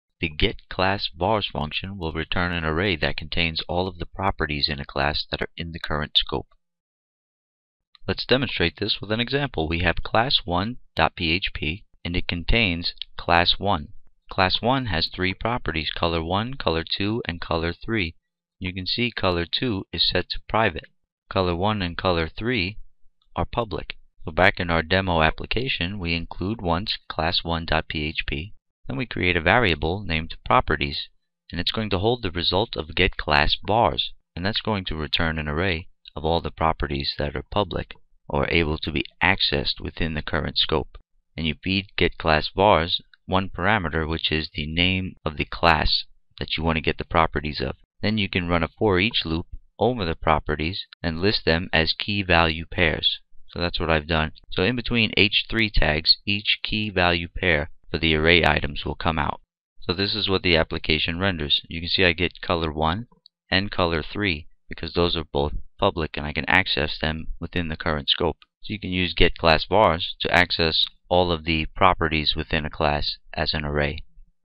PHP Tutorial - get_class_vars() Function Object Oriented Programming OOP
PHP get_class_vars() function usage demonstration.